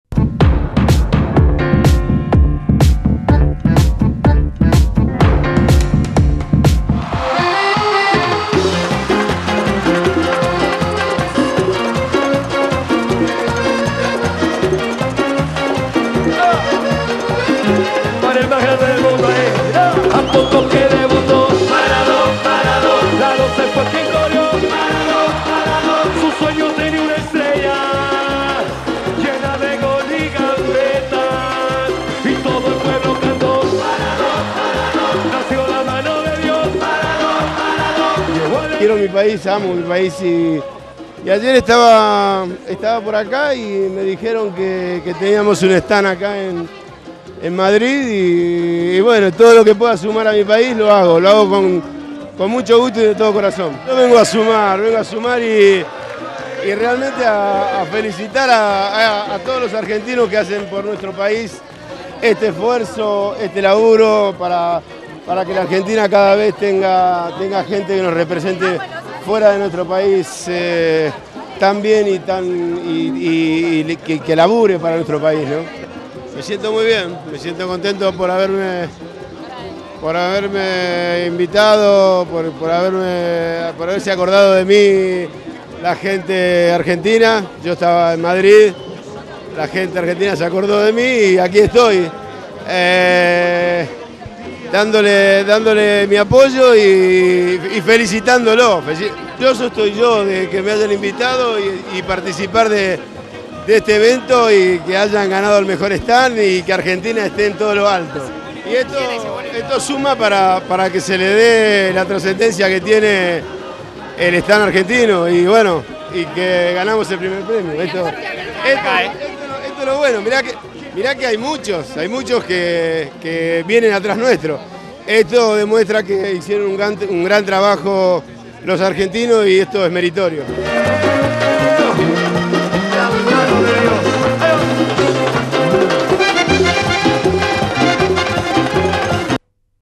Entrevista a Diego Maradona en FITUR 2008
Entrevista al futbolista argentino Diego Armando Maradona, quien visitó el stand de la Secretaria de Turismo de Argentina, en la Feria Internacional de Turismo 2008, en Madrid, España.

Una realización de Imagen Producciones
Buenos Aires, Argentina